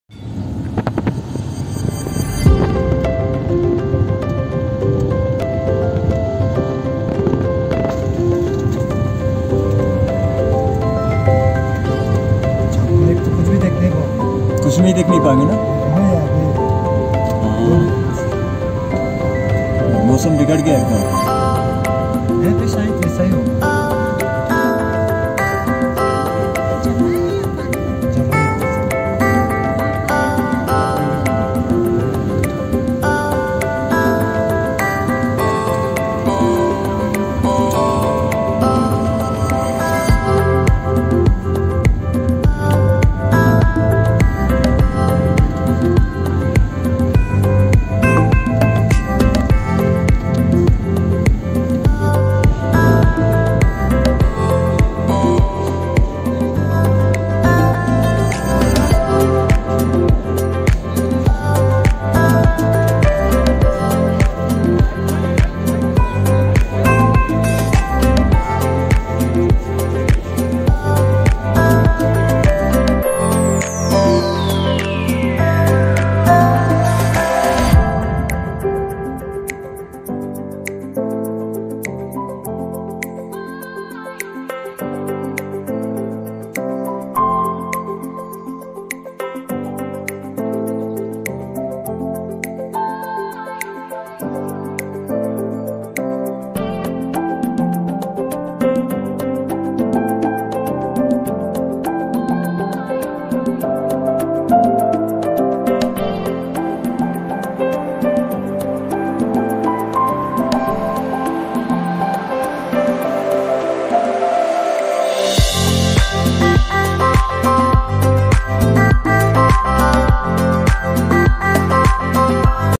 Changgu Lake, at 12,300 feet is a glacial lake in the East Sikkim ,40 kms from Gangtok
Changgu Lake, is a glacial lake in the East Sikkim some 40 kilometres from the capital Gangtok. Located at an elevation of 3,753 m (12,313 ft), the lake remains frozen during the winter season.This beautiful lake changes colour as per the season.